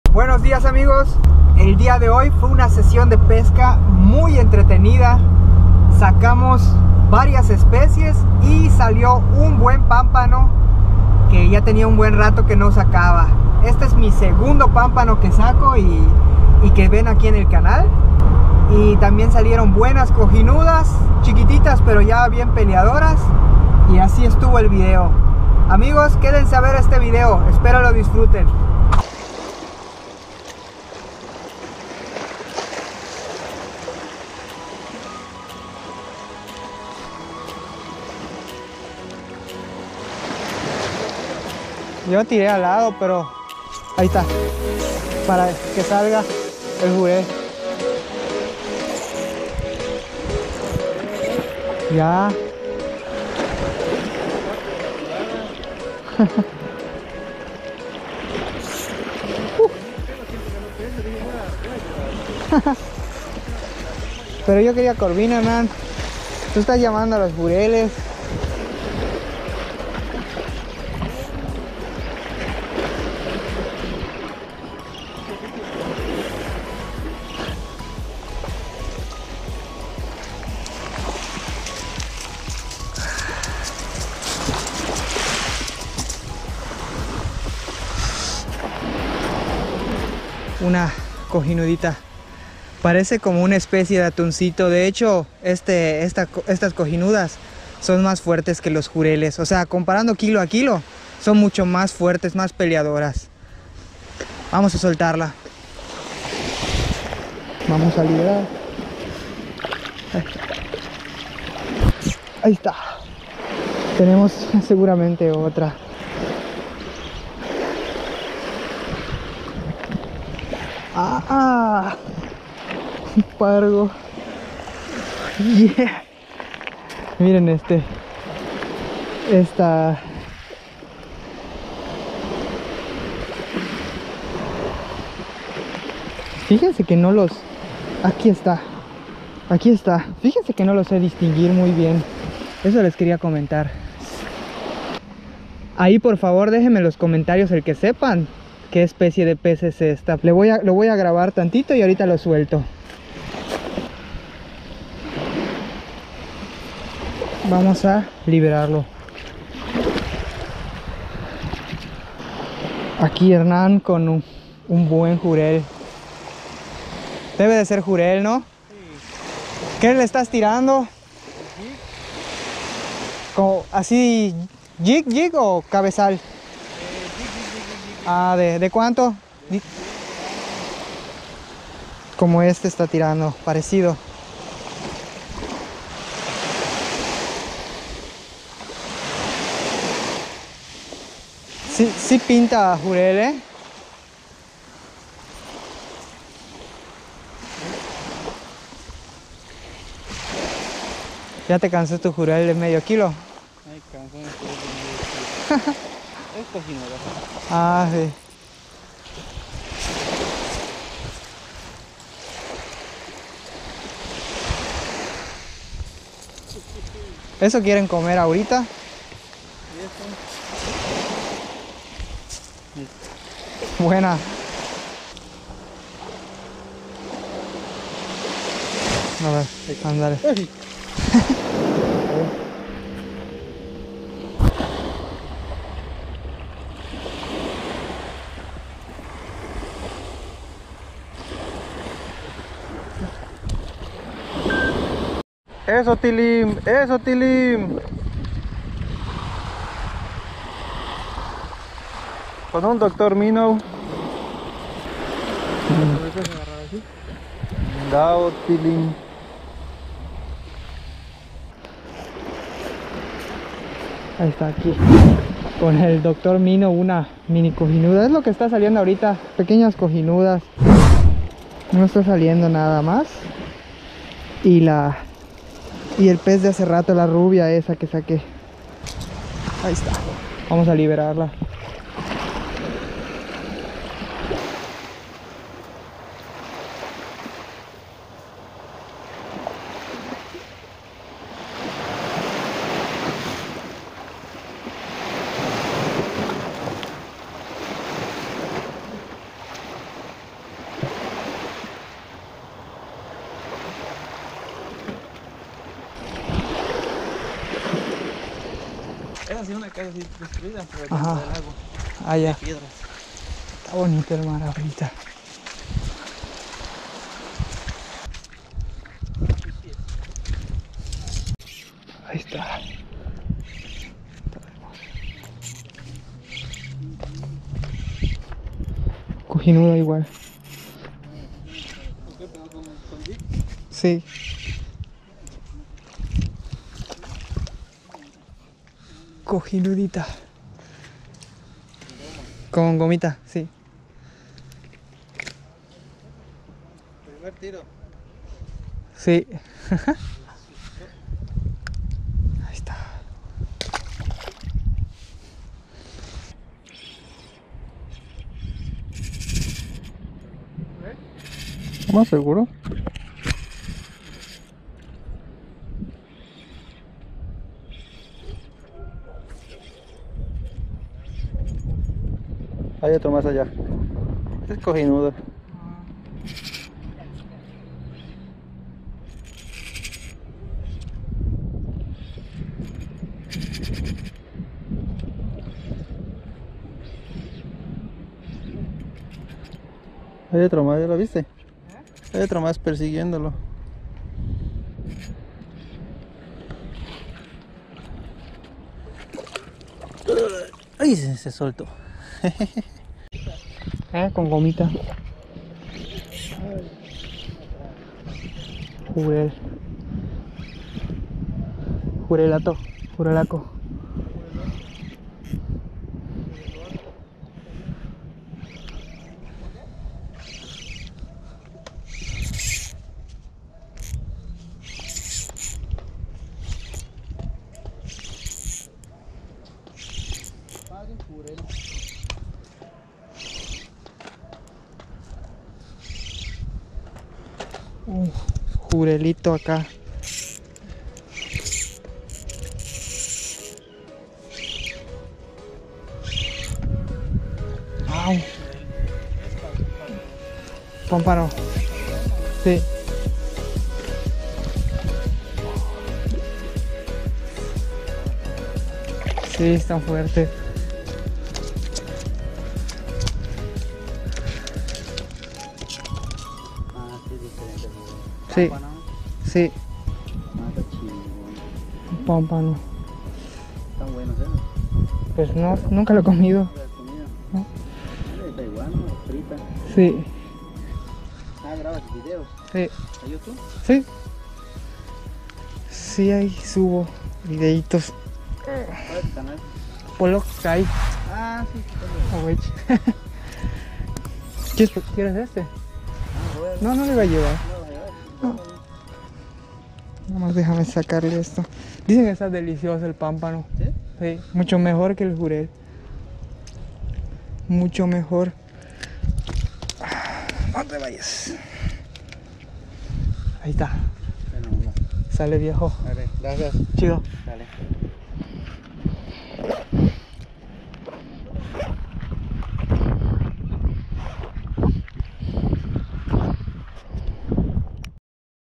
🛑MUCHA Actividad Y al final sale una Especie EXóTICA. || Cojinudas, Rubia y PáMPANO.
Que hay amigos aquí reportándonos con nuevas capturas, muy buen día y sobre todo diversión al 100, espero les guste el video.

EQUIPOS

CAÑA: kuying teton light
CARRETE: daiwa exeler tamaño 1000
LINEA: kast king trenzada 6lb
LIDER: flurocarbono 30lb

CAÑA: kastking geminus light
CARRETE: kastking sharky 3 tamaño 1000
LIDER: flurocarbono 30lb

MUSICA